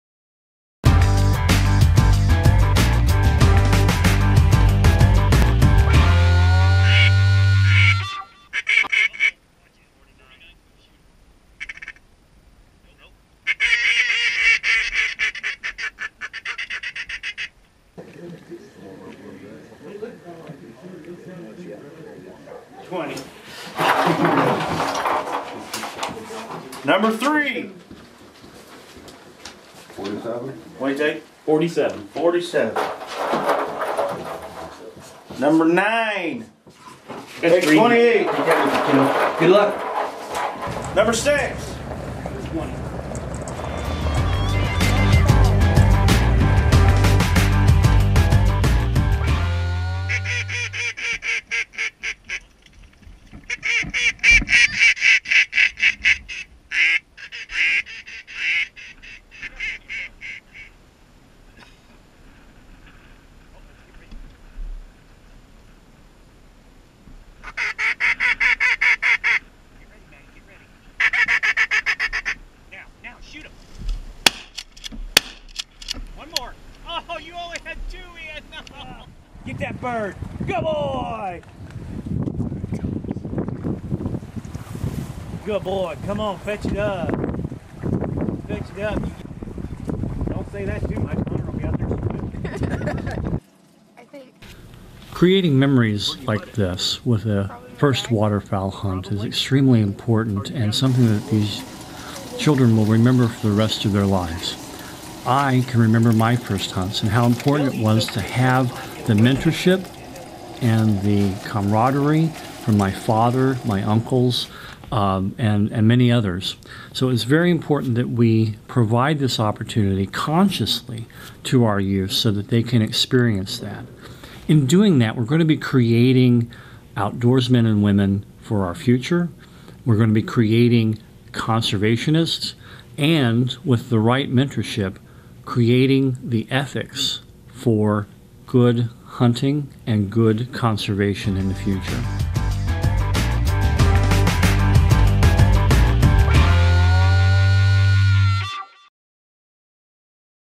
Sangchris Lake Youth Waterfowl Hunt
The Sangchris Waterfowlers Association organized the youth waterfowl hunt at Sangchris Lake State Park on Oct. 18, 2014. About 25 young hunters participated. Special thanks to all the volunteers who made the hunt fun and successful.